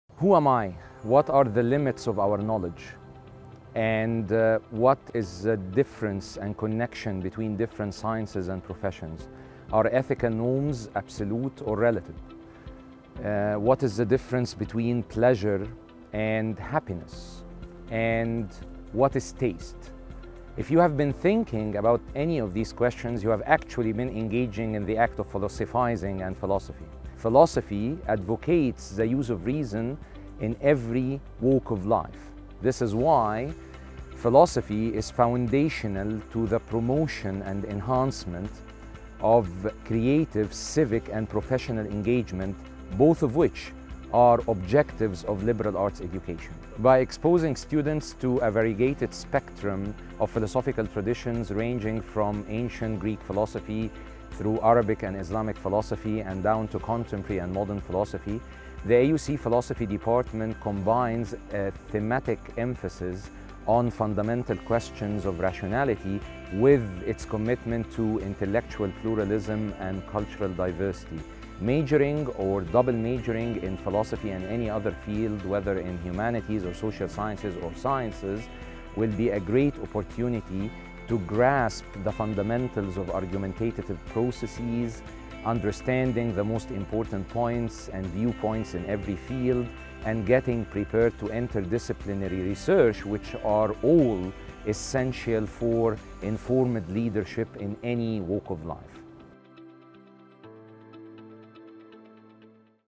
Philosophy Major
Assistant professor, Ahmed Abdel Meguid '02,  gives a short brief about AUC’s philosophy undergraduate program; what students can expect to learn, the hands-on experiences offered, and the career paths students can take on upon graduation.